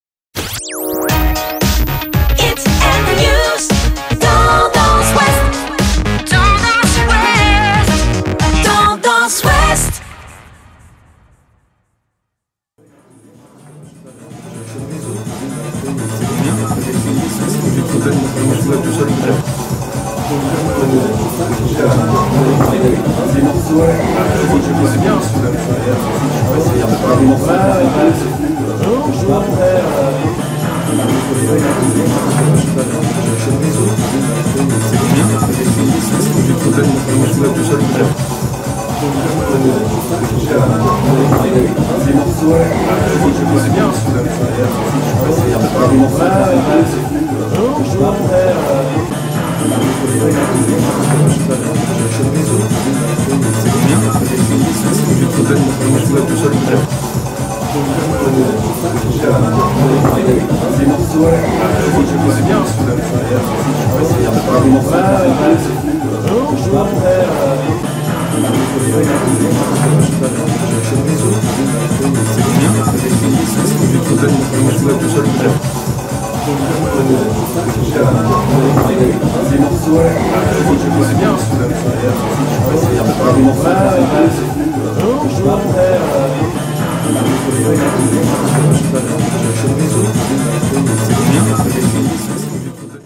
Le Disquaire Day célèbre le disque vinyle à Saint-Lô
Disquaire Day... la fête du disque vinyle se déroule ce samedi 20 avril chez les disquaires indépendants dans la région.